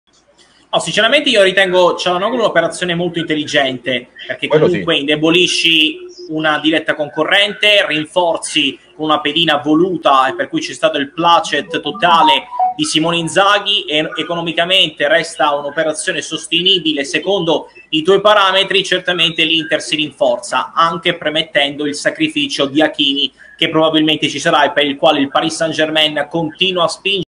Calhanoglu all’Inter, operazione intelligente
L’Inter prende Hakan Calhanoglu e regala a Simone Inzaghi una pedina funzionale e di qualità